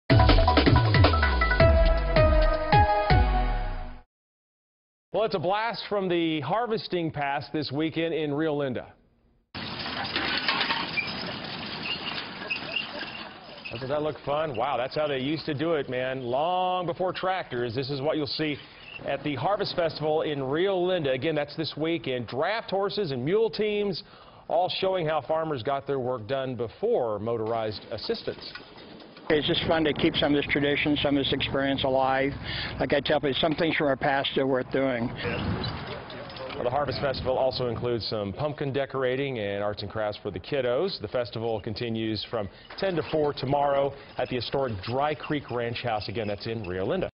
Blast from harvesting past in Rio Linda
The Harvest Fest this weekend in Rio Linda will show you how they used to farm before tractors.